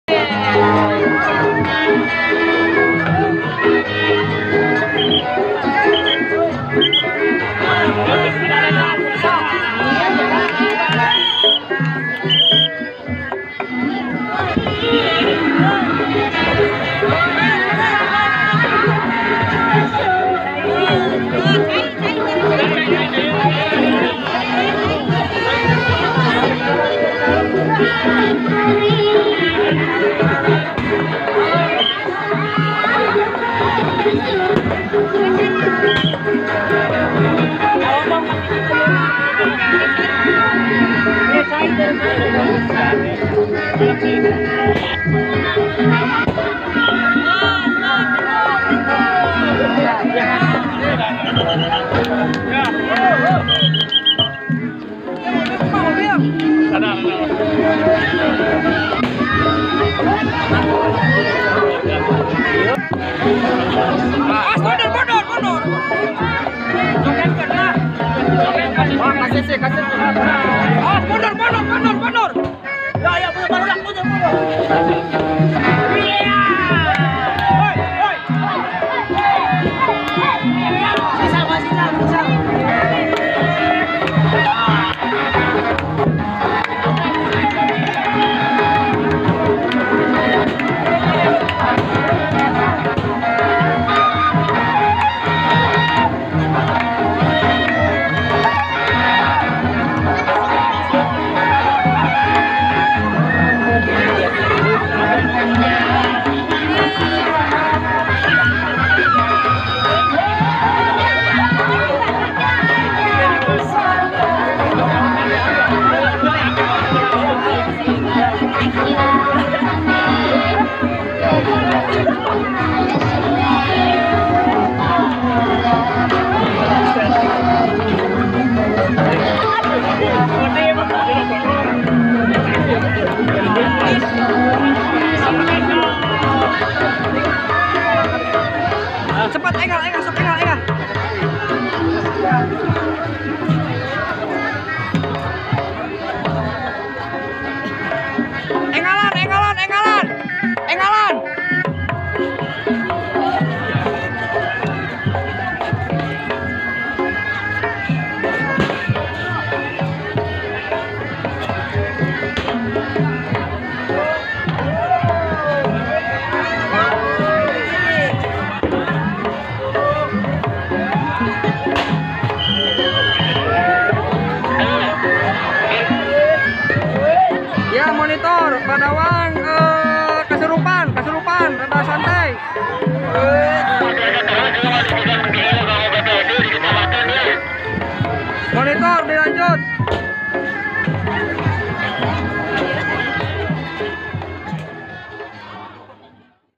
Badawang Pasirwaru Kasurupan
Garut, Seni dan Budaya badawang pasirwaru yang selalu ditunggu-tunggu warga Limbangan kini tampil dengan empat buah badawang. Selain itu, salah satu personil Bsdawangpun kerasukan leluhur Badawang.